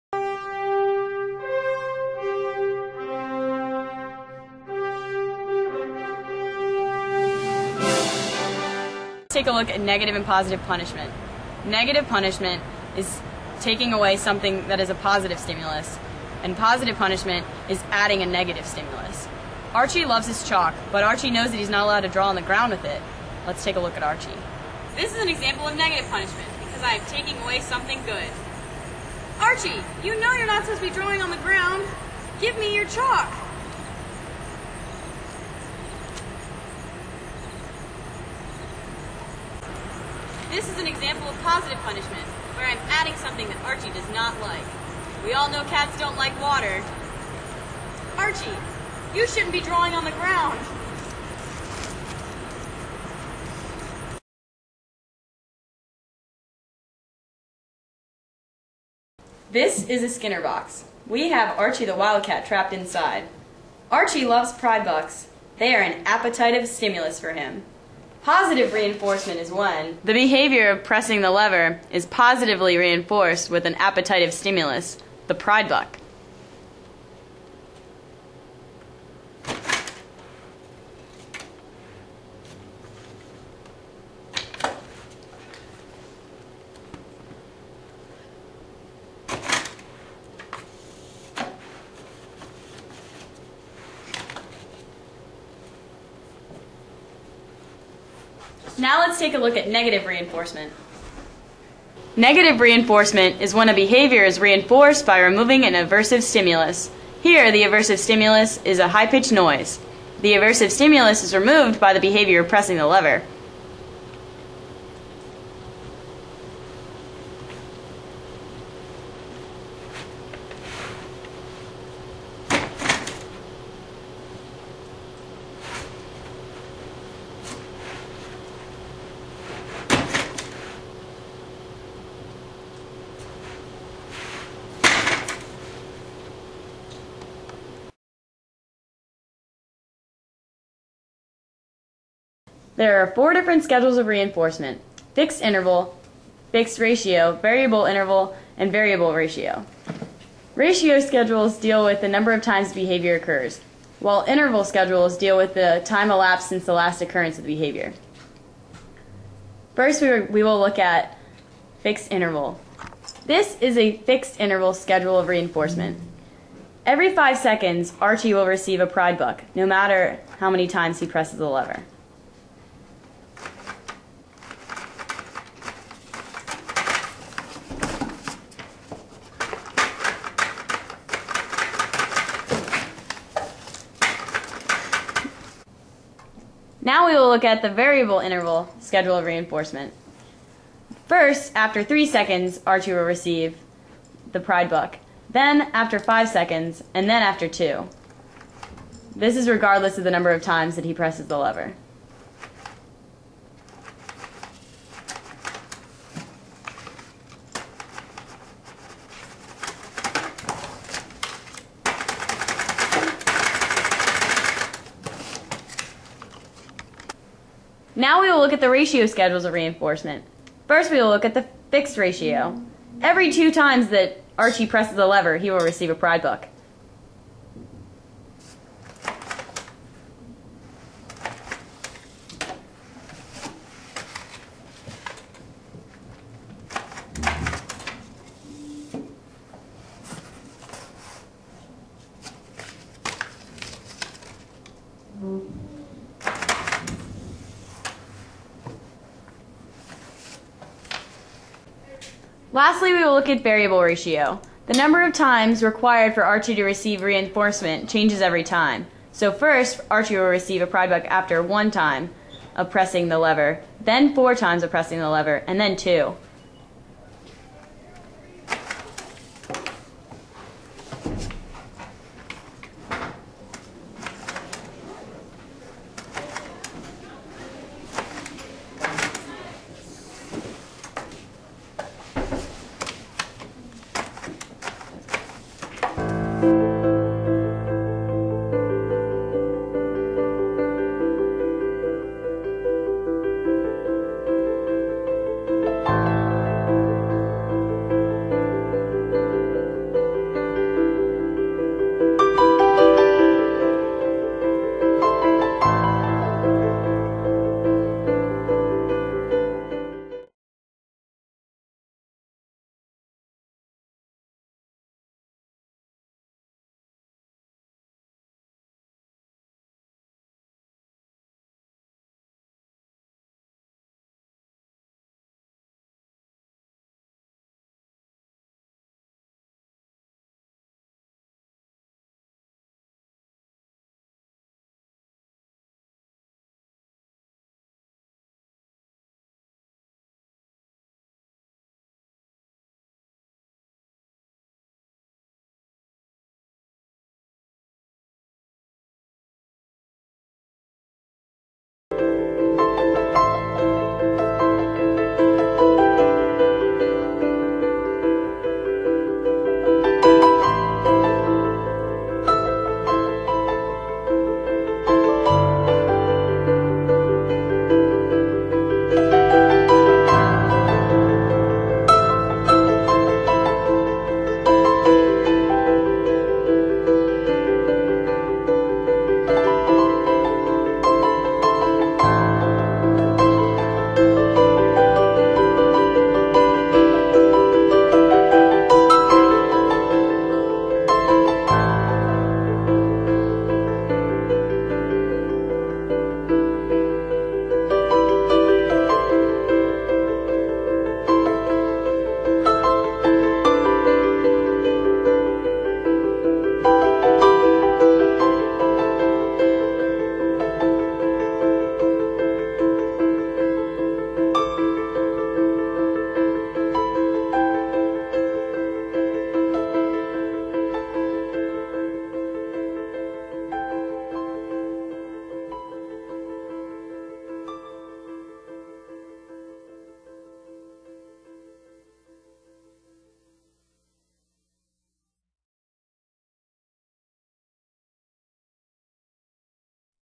Operant Conditioning: The skinner box
This video was done by my independent study students at Arundel HS.  It shows the various types of reinforcement and punishment associated with Operant Conditioning.